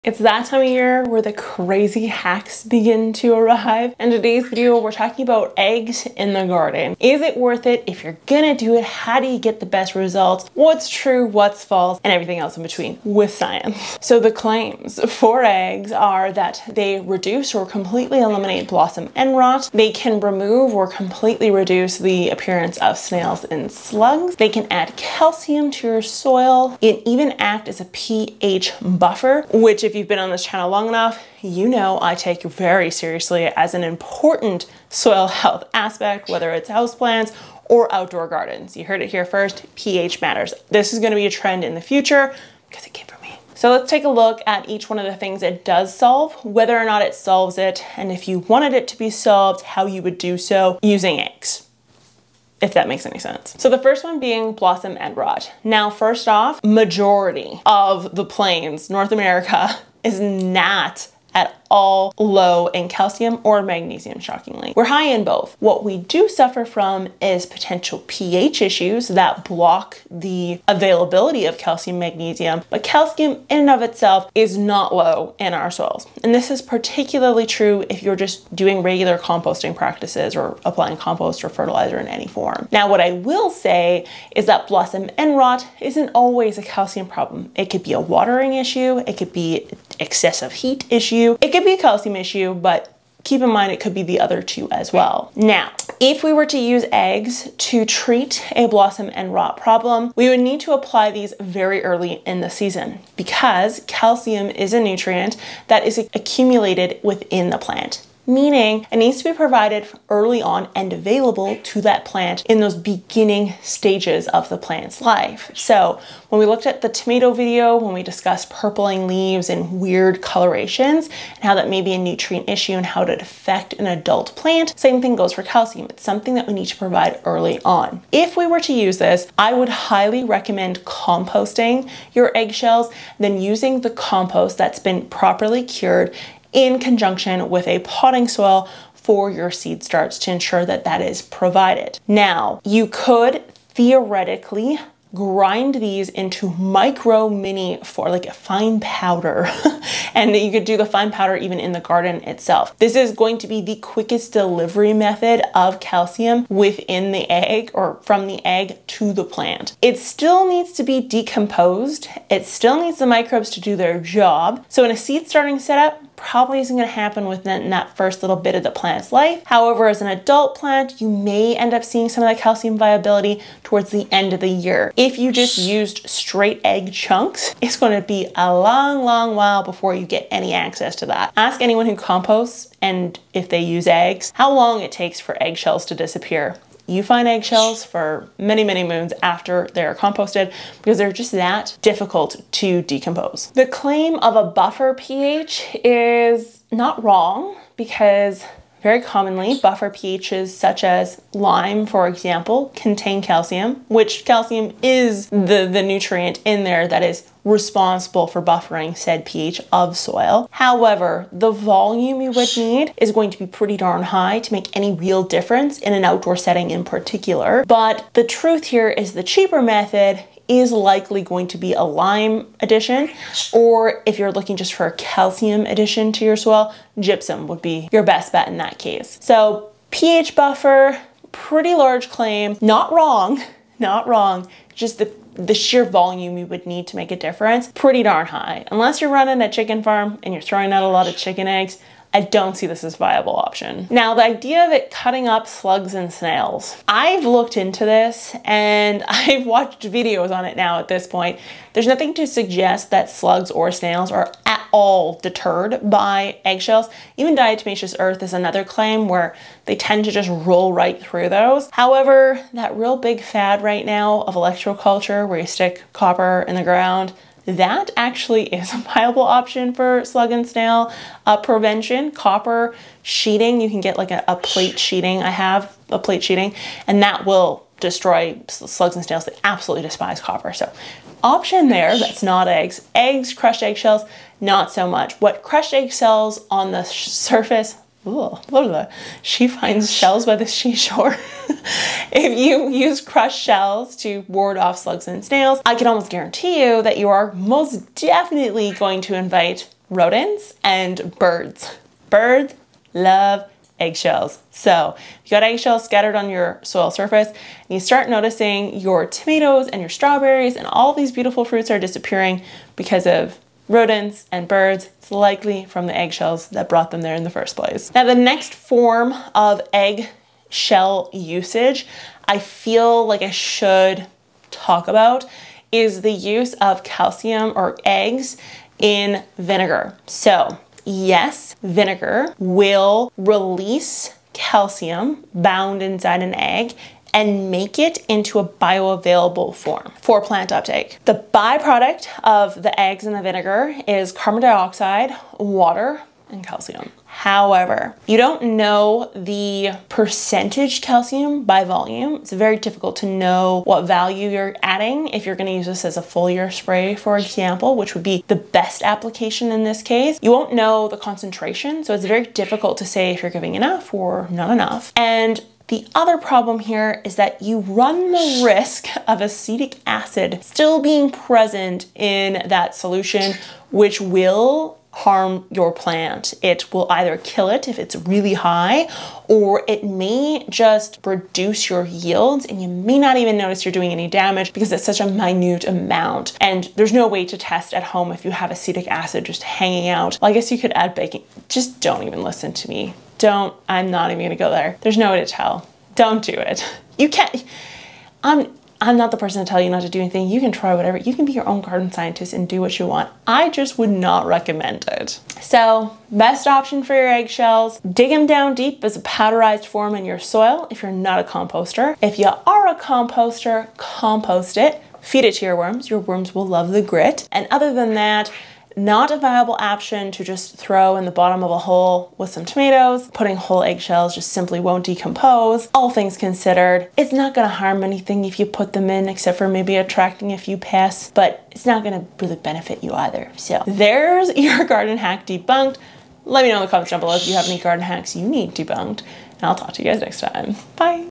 Why Egg Shells In The Garden Will NOT Solve Your Problem.
Egg shells for garden soil are supposedly able to cure blossom end rot, adjust ph and even get rid of slugs! But is there any science to back the use of eggs in the garden? If yo I have issues with birds and rodents the use of egg shells in your soil may cause problems. Let look at the Truth About EGG Shells In The Garden.

Paperback Houseplant Planner:
Paperback Garden Planner:
Digital Download Planners